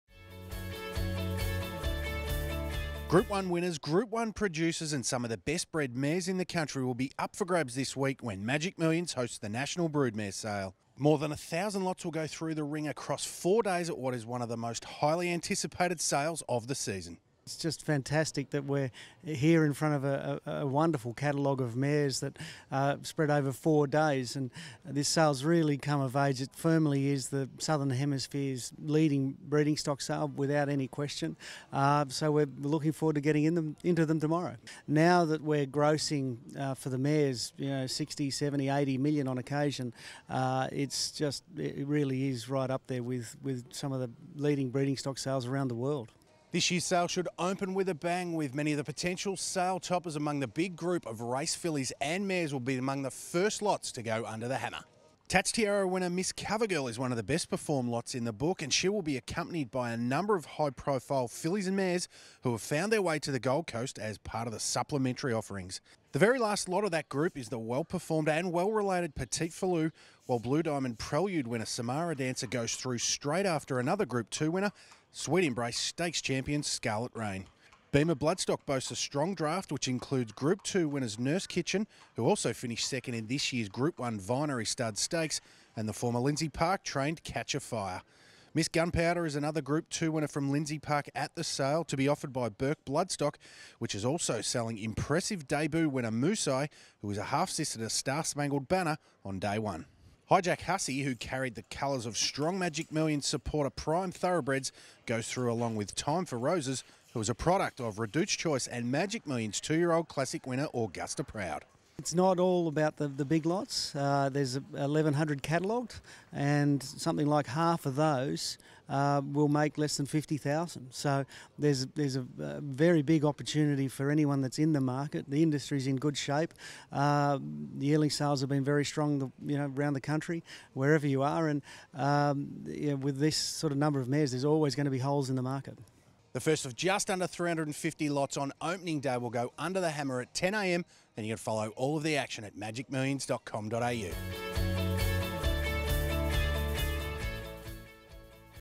2017 Magic Millions National Broodmare Sale Preview
The world's bloodstock focus is on the Gold Coast for the 2017 Magic Millions National Broodmare Sale.